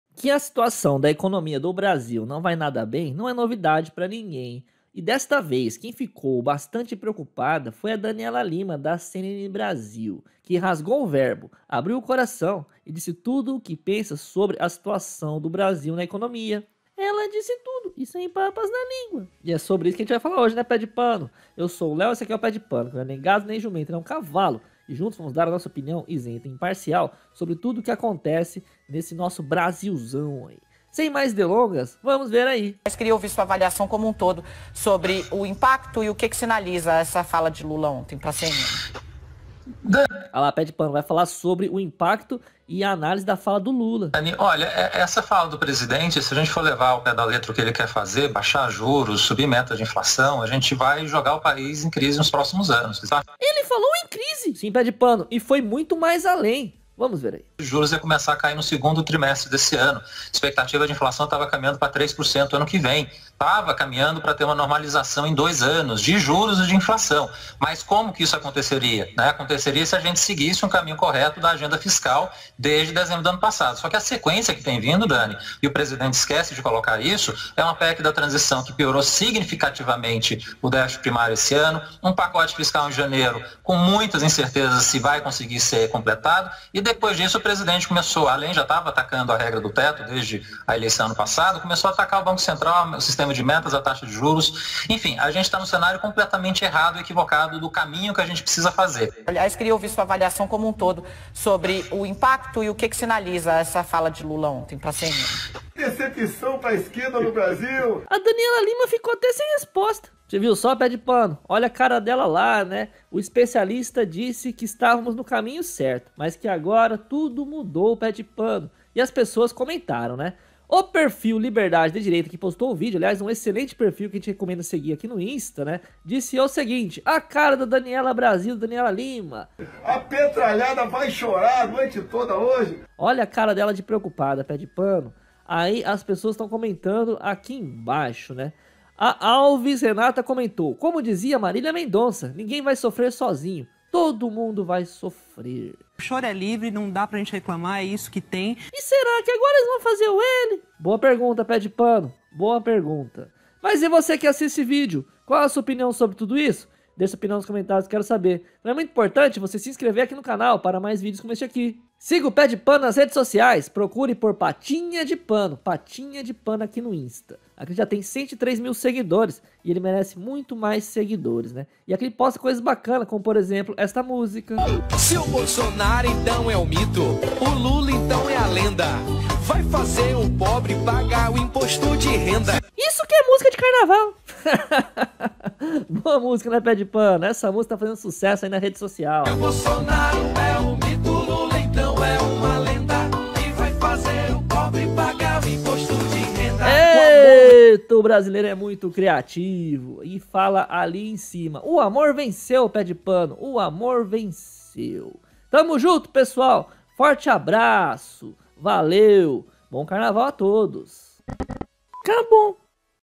NOTÍCIA RUIM DEIXA CLIMA DE PREOCUPAÇÃO ENTRE JORNALISTAS DA CNN BRASIL
Essa notícia ruim da economia deixou jornalistas da CNN Brasil em clima de preocupação